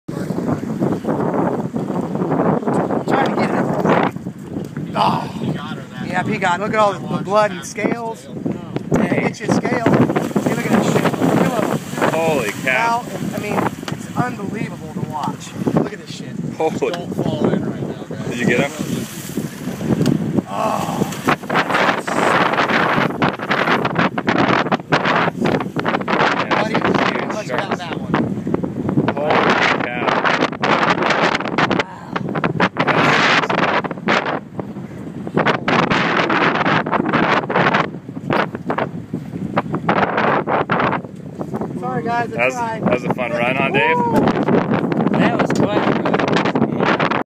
Shark Attack Boca Grande - Tarpon Attacked by Bull Sharks
This was a major bummer. This is the first Tarpon I was able to hook and it looked like around 175llbs+. We were able to help the Tarpon avoid the first shark attack by letting it run ashore. Unfortunately the Tarpon bolted again this time there were two massive Bull Sharks waiting for it.